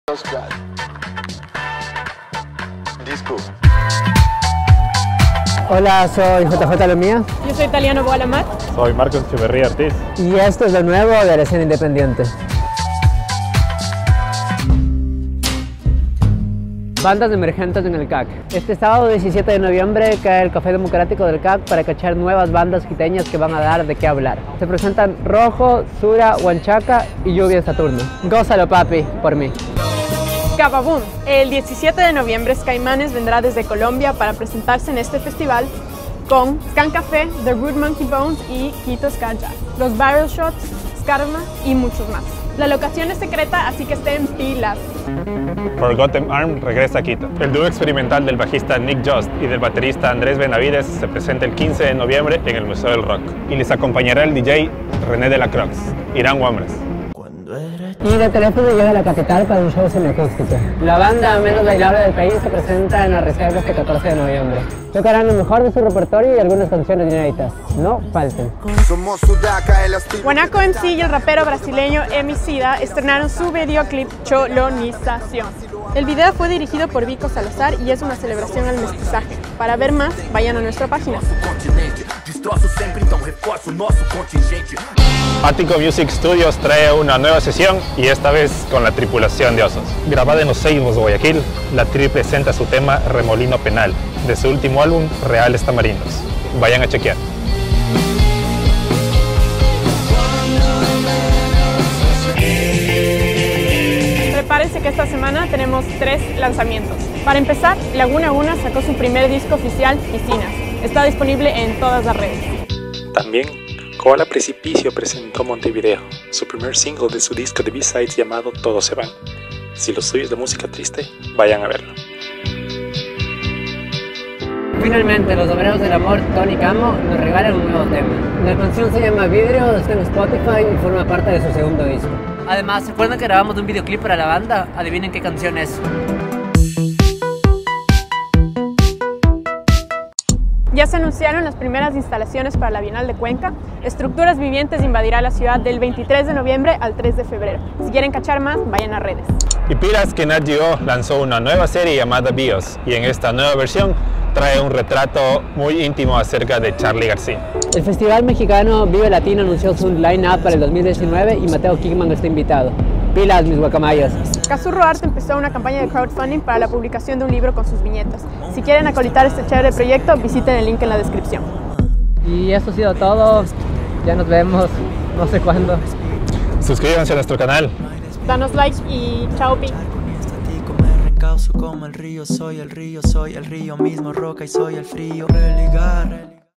LO NUEVO RC - Bandas Emergentes, Ska-Ba-Boom, Letelefono acústico
LoNuevoRC visita Brooklyn

FAN FACTS:

+ Cazurro / Crowdfunding para Dejá Vu